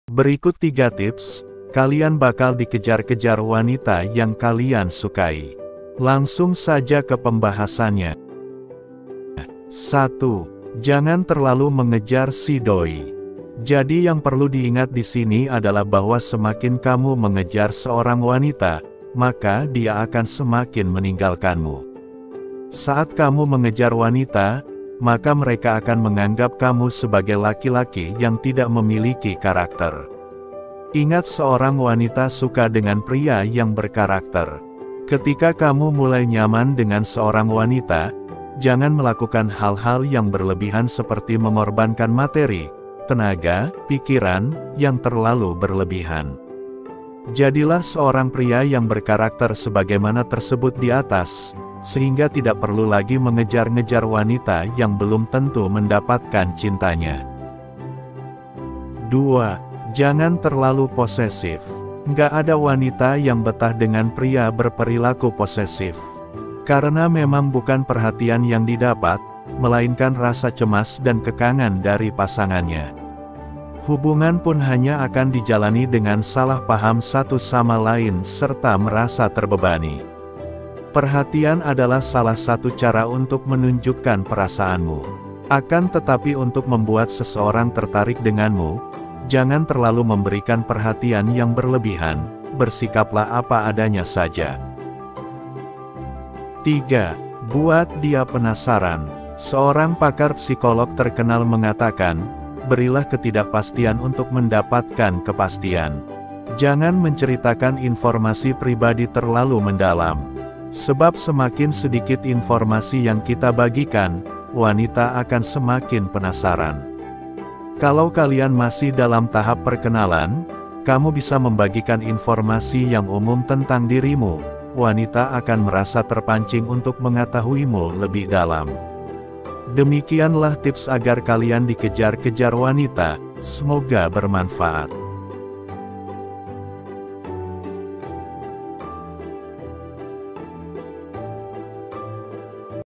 3 TIPS AGAR KALIAN DIKEJAR KEJAR WANITA - cara agar cewek tergila-gila
Berikut 3 tips,  kalian bakal Dikejar-kejar Wanita yang kalian sukai.

Langsung saja ke pembahasannya.

1.Jangan Terlalu Mengejar si Do'i .

Jadi yang perlu diingat disini adalah bahwa semakin kamu mengejar seorang wanita, maka dia akan semakin meninggalkanmu.

saat kamu mengejar wanita, maka mereka akan menganggap kamu sebagai laki-laki yang tidak memiliki karakter.
Ingat seorang wanita suka dengan pria yang berkarakter.

Ketika kamu mulai nyaman dengan seorang wanita, jangan melakukan hal-hal yang berlebihan seperti mengorbankan materi, tenaga, pikiran, yang terlalu berlebihan.

Jadilah seorang pria yang berkarakter sebagaimana tersebut diatas, sehingga tidak perlu lagi mengejar-ngejar wanita yang belum tentu mendapatkan cintanya.

2.Jangan Terlalu posesif.

Nggak ada wanita yang betah dengan pria berperilaku posesif,
Karena memang bukan perhatian yang didapat, melainkan rasa cemas dan kekangan dari pasangannya.
Hubungan pun hanya akan dijalani dengan salah paham satu sama lain serta merasa terbebani.

Perhatian adalah salah satu cara untuk menunjukkan perasaanmu.

Akan tetapi Untuk membuat seseorang tertarik denganmu, jangan terlalu memberikan perhatian yang berlebihan, bersikaplah apa adanya saja.

3.Buat Dia Penasaran.

Seorang pakar psikolog terkenal mengatakan, “Berilah ketidakpastian untuk mendapatkan kepastian.”

Jangan menceritakan informasi pribadi terlalu mendalam,
Sebab semakin sedikit informasi yang kita bagikan, wanita akan semakin penasaran. Kalau kalian masih dalam tahap perkenalan, kamu bisa membagikan informasi yang umum tentang dirimu,  wanita akan merasa terpancing untuk mengatahuimu lebih dalam.

Demikian lah tips agar kalian dikejar kejar wanita, smoga bermanfaat.